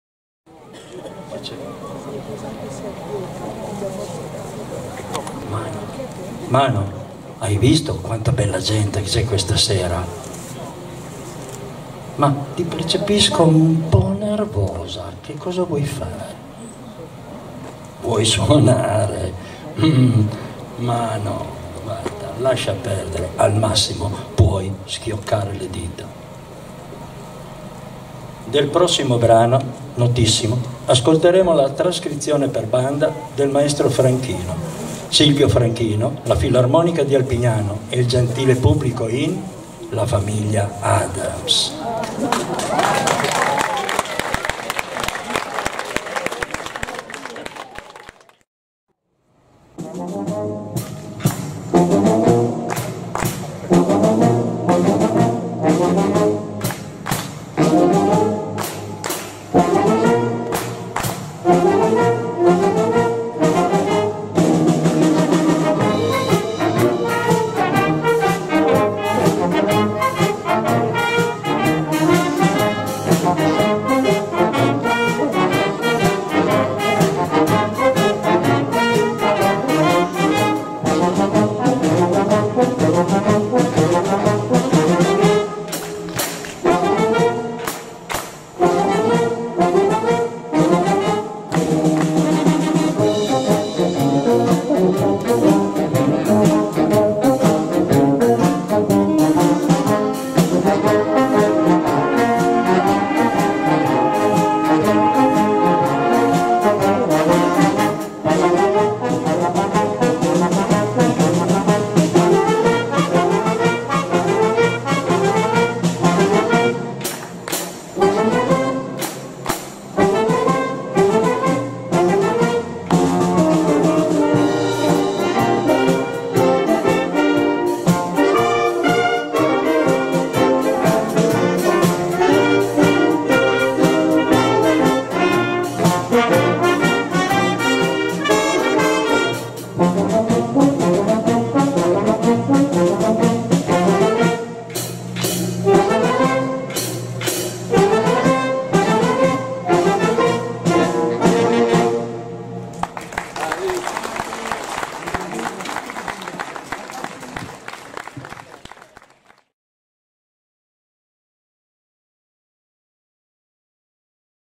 Filarmonica di Alpignano concerto San Giacomo 2022 brano quarto
Filarmonica di Alpignano concerto San Giacomo 2022
brano quarto
dirige il
maestro Silvio Franchino